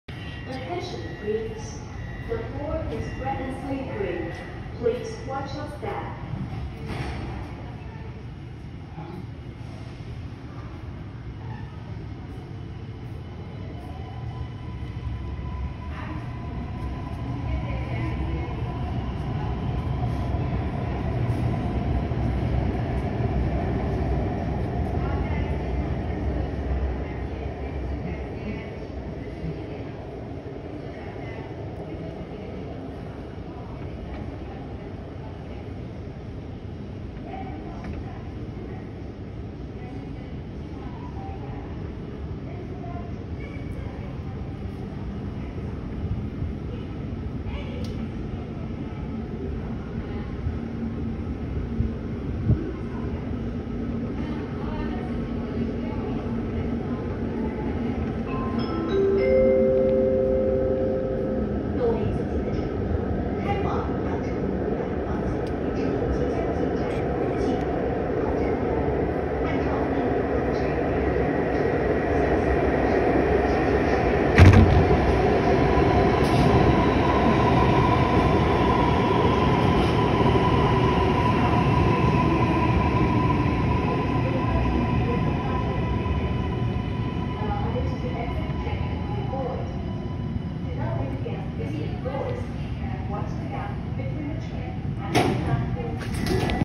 Train approaching [Suzhou Rail Transit Line 5/Trial Run]
The video was taken from Xingtangjie(星塘街) station.
The train is bound for Yangchenghu South(阳澄湖南) Station.
Video was taken during the trial run under the permission.